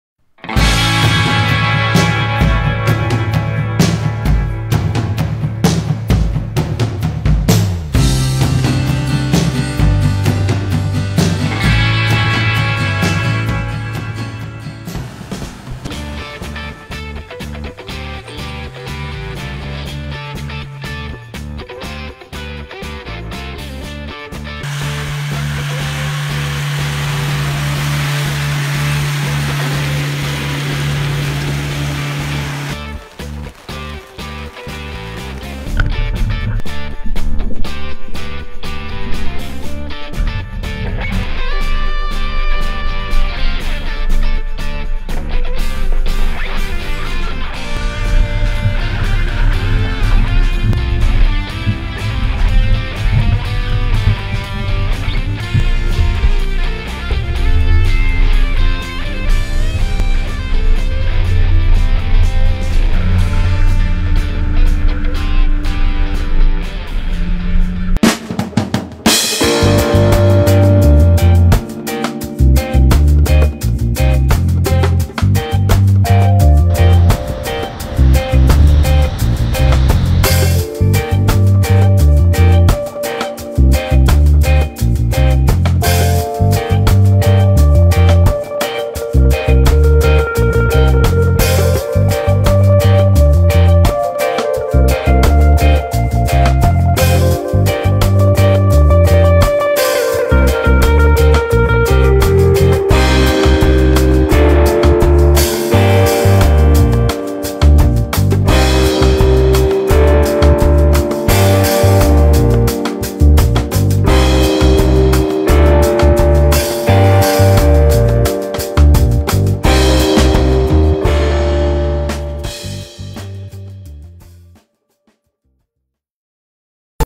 Volcano Off Road Camper: El Camper para los Caminos de Chile
Conoce nuestros novedosos modelos de campers off road, diseñados con los estandares del mercado australiano, el más exigente y avanzado del mundo en off road.